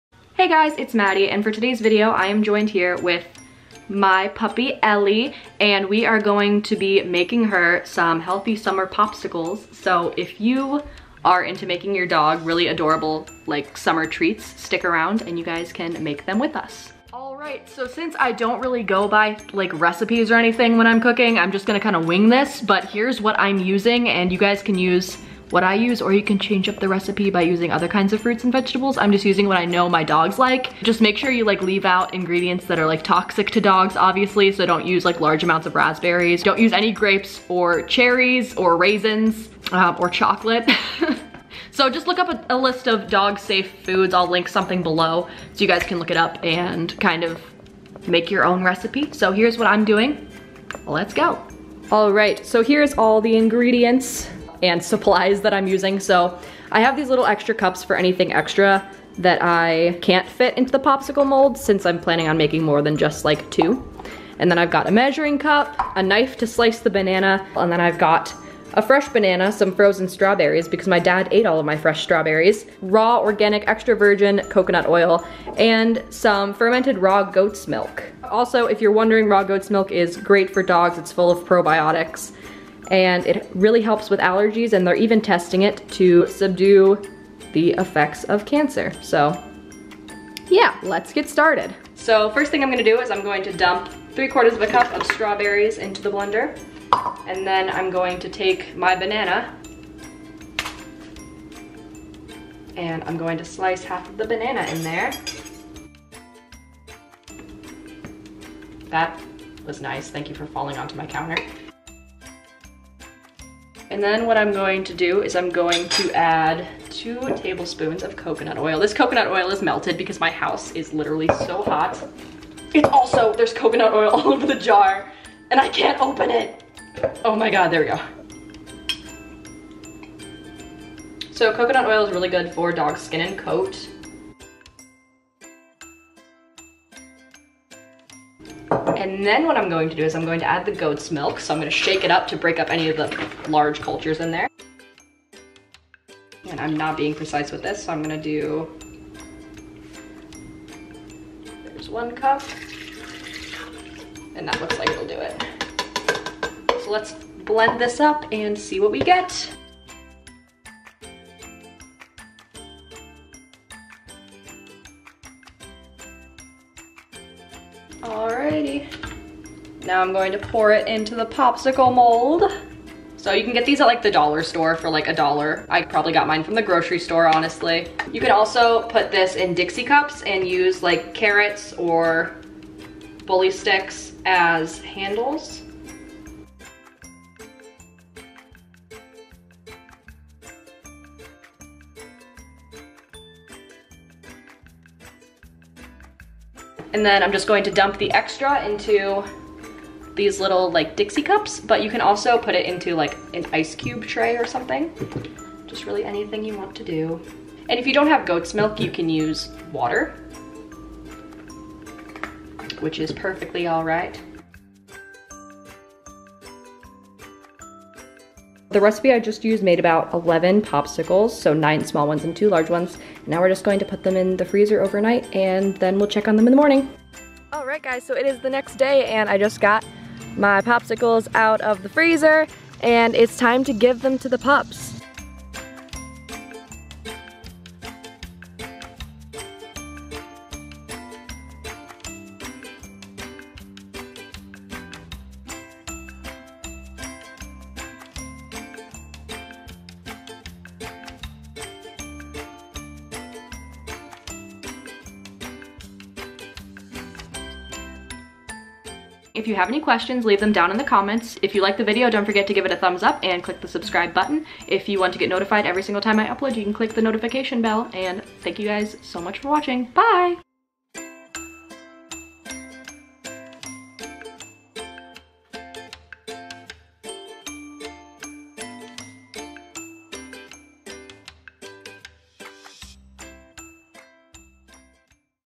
DIY HEALTHY SUMMER PUPSICLES / MAKE POPSICLES FOR YOUR DOG! (Raw Goat Milk)
WRITE TO ME! ~ I will write you back!